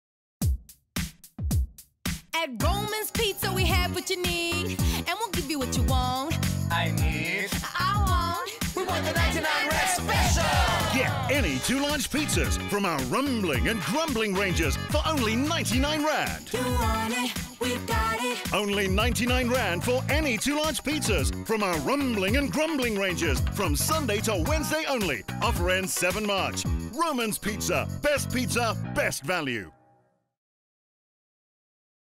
Roman's Pizza R99 Special | 2 Large Pizzas For R99 | January 2018
Get Any Two Large Pizzas From Our Rumbling & Grumbling Ranges For Only R99. Choose From 16 delicious Flavours. Offer Valid Every Sunday - Monday 14 January - 07 March 2018. T&Cs Apply.